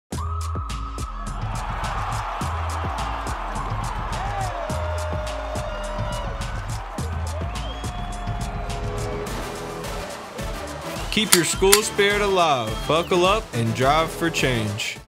Keep Your School Spirit Alive: Buckle Up, Slow Down, and Drive for Change- 15 second
YOVASO is happy to work with WFXR on this exciting PSA for the 2021 Drive for Change: Buckle Up and Slow Down Campaign. The talents in the PSA are students at Northside High School in Roanoke, VA. Their names are: Siya Bhagat, Morgan Doyle, Allison Delareza, Vy Chau, Simone Hamlar, Andrew Binns-Loveman, and Maryn Kroeger. The voiceover talent is Bailey Palmer, a 2021 graduate of William Byrd High School in Vinton, VA. This 15 second spot will run as an ad on TikTok during YOVASO's fall campaign.